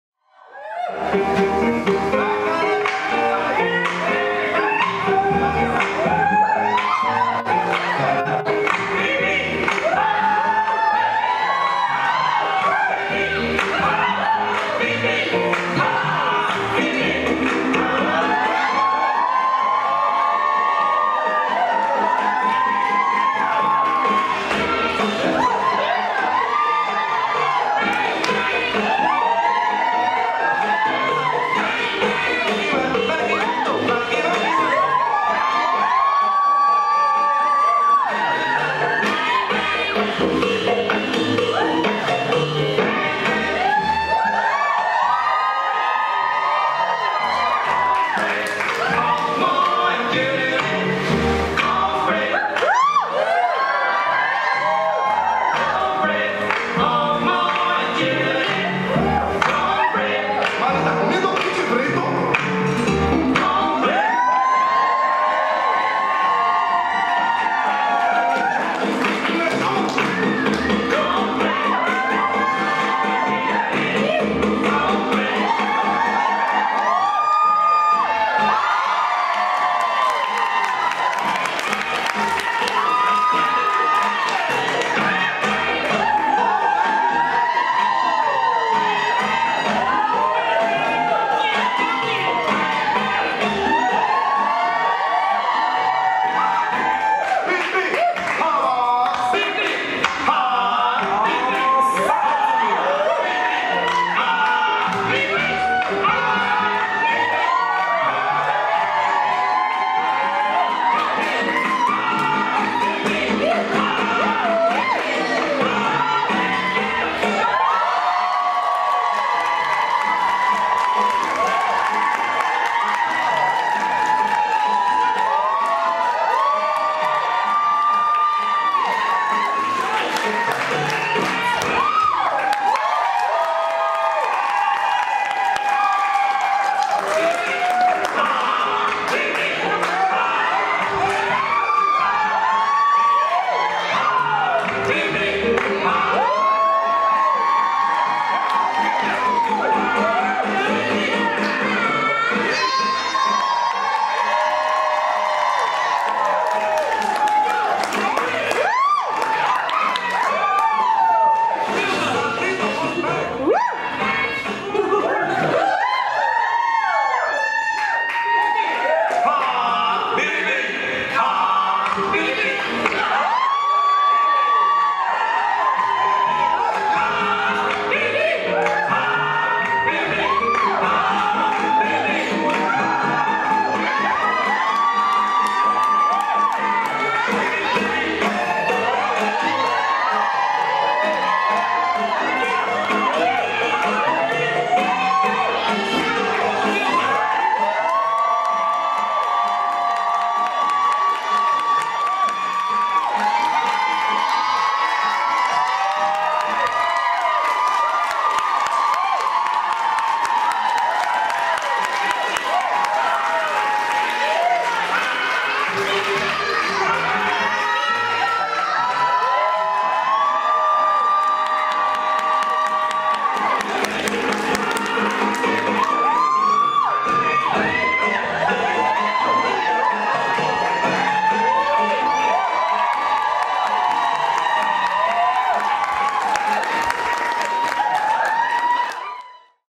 Diversion Burlesk presents Pasties & Pastries: Titty Titty Bang Bang
This day time show on January 26, 2020 gave performers the opportunity to have their performances recorded to a live audience, and the audience an opportunity to not have to be out late on a Sunday night!

Filmed at the Victoria Event Centre in Victoria, BC, this show was delicious in more ways than one!

One of those ways was Titty-Titty Bang-Bang! This ex-pat Canadian has re-repatriated in the best way and is gracing our stages once again. Always a trick up her sleeve, she will leave you screaming!